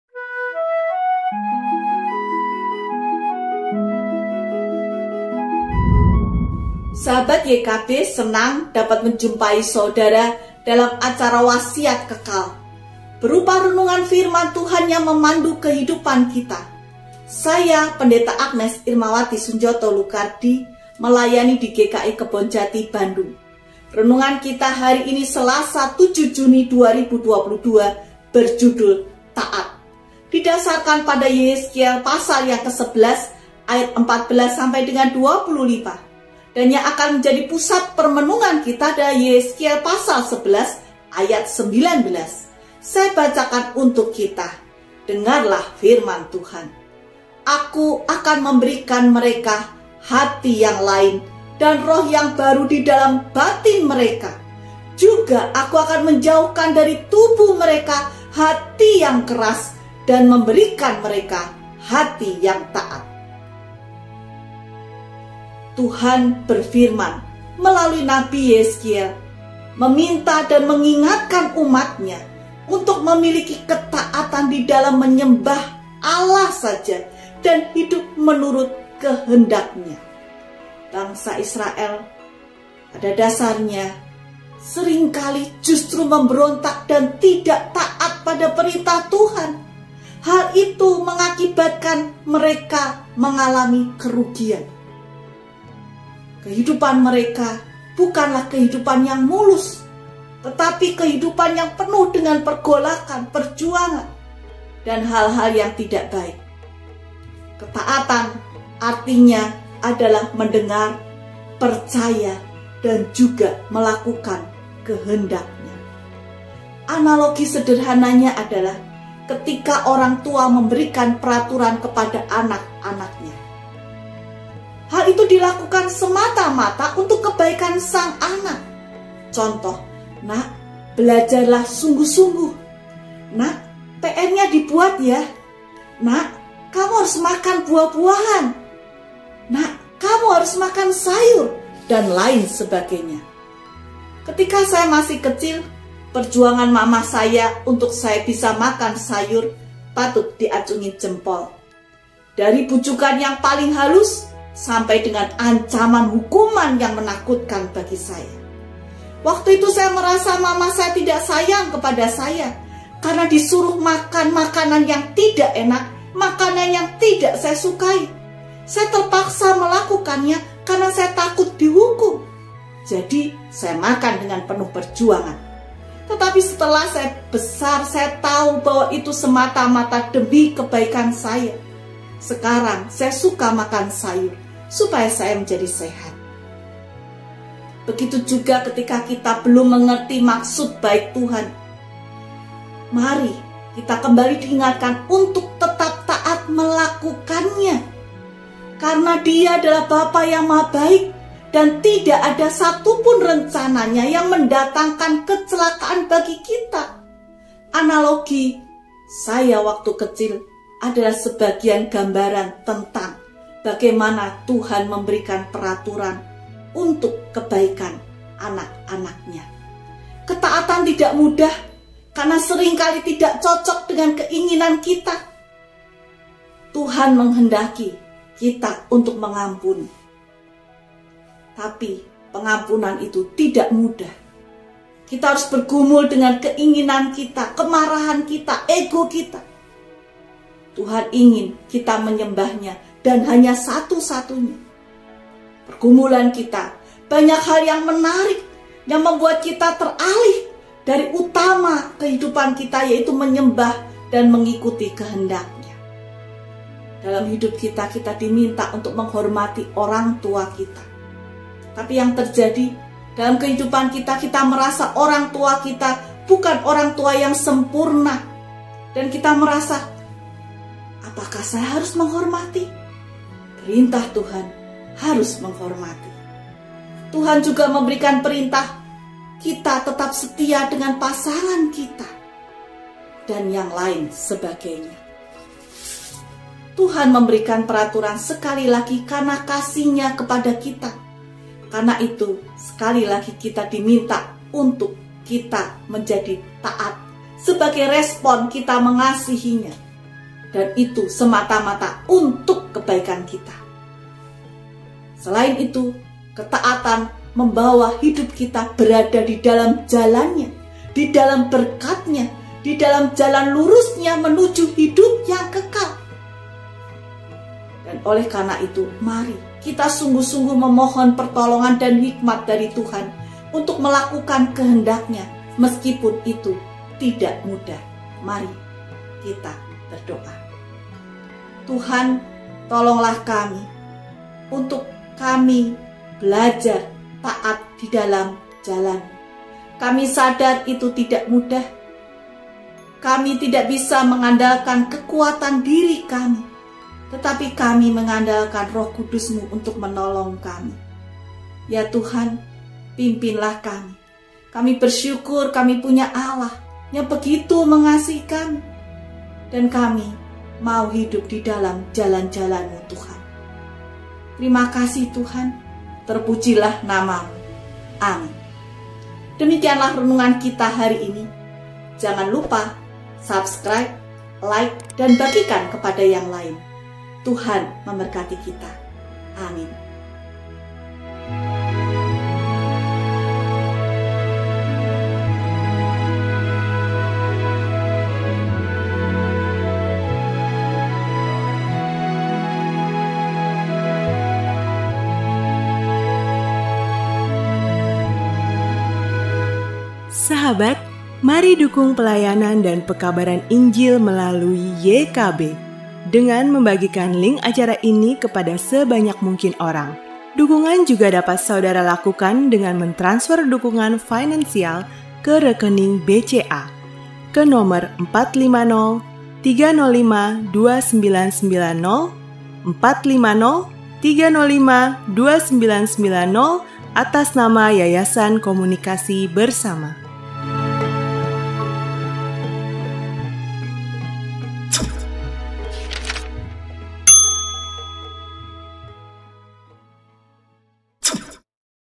WASIAT KEKAL 464 - TAAT!
Renungan Harian “Wasiat Kekal” untuk hari ini, Selasa, 7 Juni 2022,
berjudul : Taat!
Didasarkan pada : Yehezkiel 11 : 14 - 25
Dibawakan oleh : Pdt. Agnes Irmawati Sunjono Lukardie, dari GKI Kebon Jati Bandung.